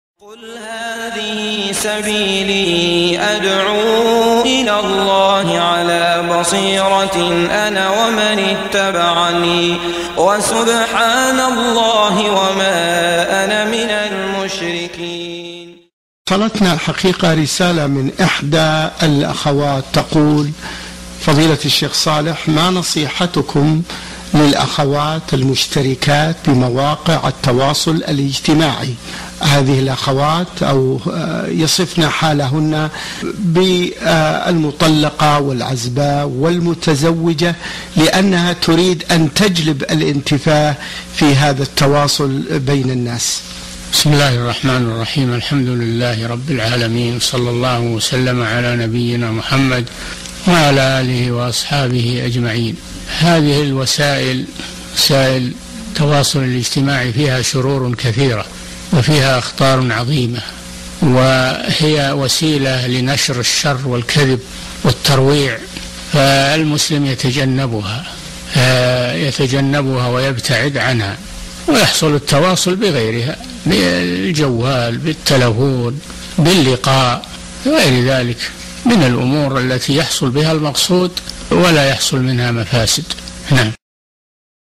Advice to the Sisters who use Social Media - Shaikh Salih Al Fawzan
Questioner: A letter has reached us from one of the sisters saying:  oh Virtuous Shaikh Salih: What is your advice for sisters who participate on social media networks? These sisters describe their situation as divorced or unmarried or married and they want to bring benefit by connecting with the people?